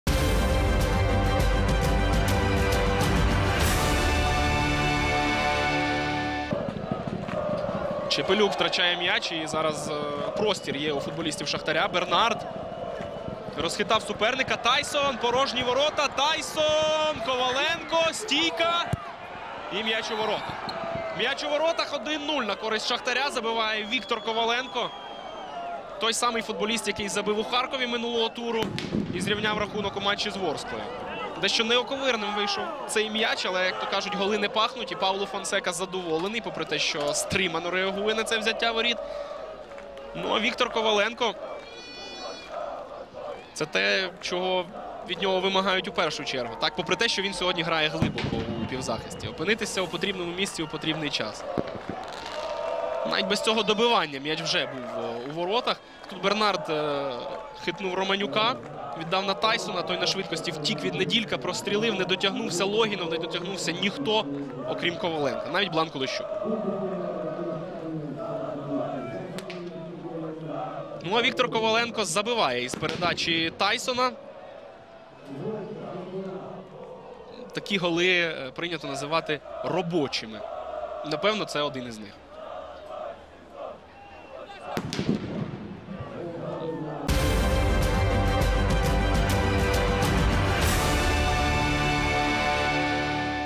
Волинь - Шахтар - 0:1. Відео голу Коваленка
66 хвилина. Гол. Бренард віддає в штрафну Тайсону, той прокинув м'яч повз Неділька і встиг майже з лінії воріт віддати назад на Коваленко, Віктор зумів штовхнути м'яч у дальній кут - той потрапляє в штангу і летить уздовж лінії воріт, можливо, вже і повністю перетнув, але для гарантії Віктор забиває його в ворота.